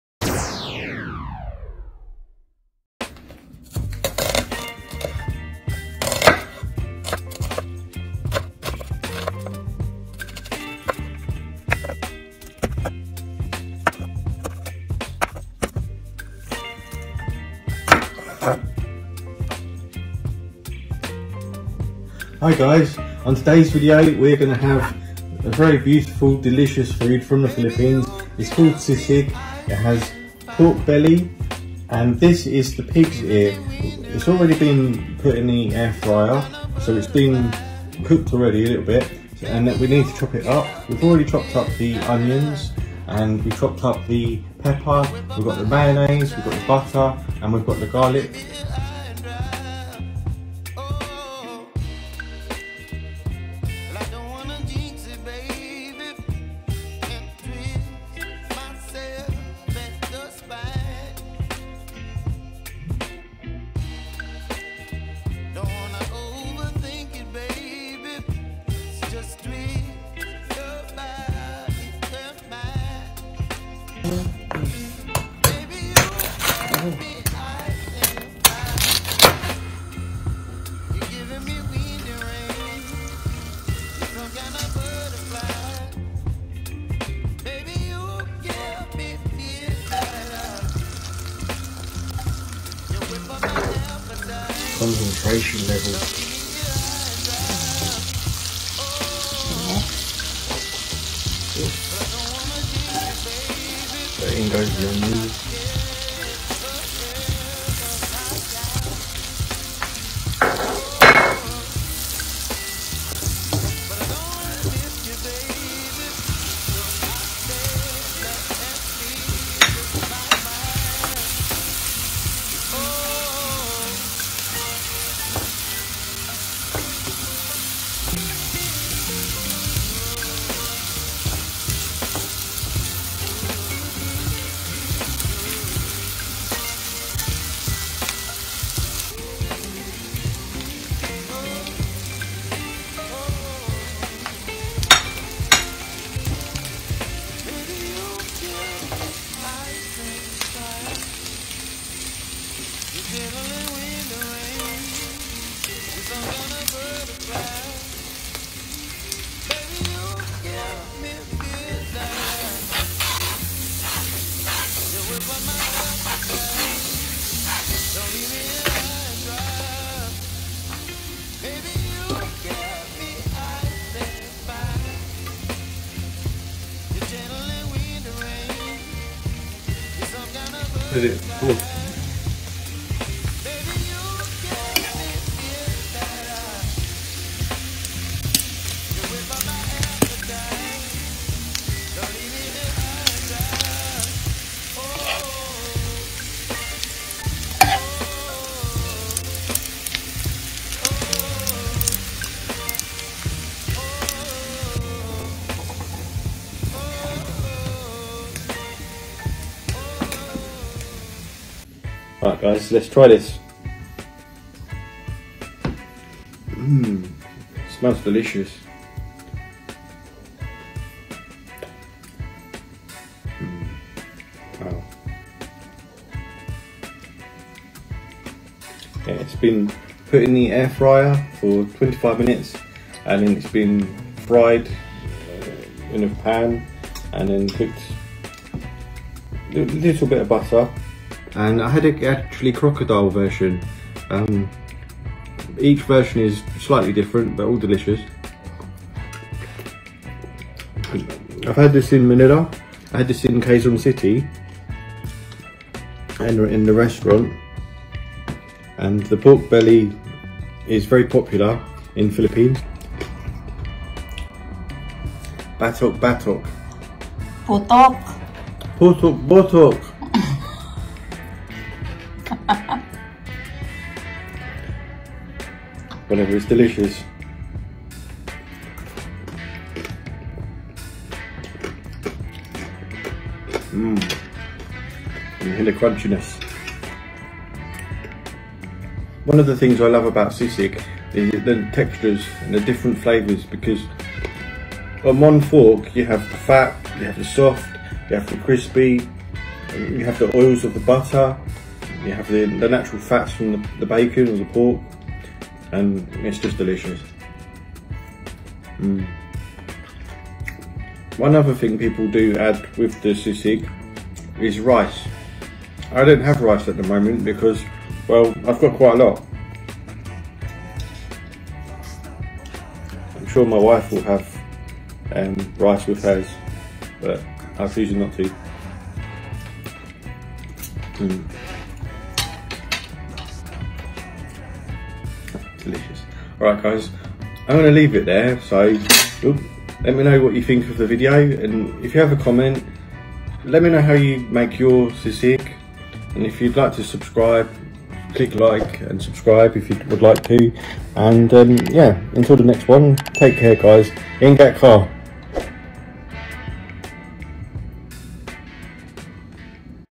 My wife cooked Sisig for the first time | Filipino Pork sisig recipe
My wife cooked Filipino Pork Sisig for the first time in UK.

This video shows how to cook sisig using pork belly and ears. Sisig is a popular dish in the Philippines. It can be consumed as a main dish with rice. It also makes a good pulutan. We really enjoy this dish! Masarap!

Mylakbay: Food and Travel Videos : My wife cooked Sisig for the first time | Filipino Pork sisig recipe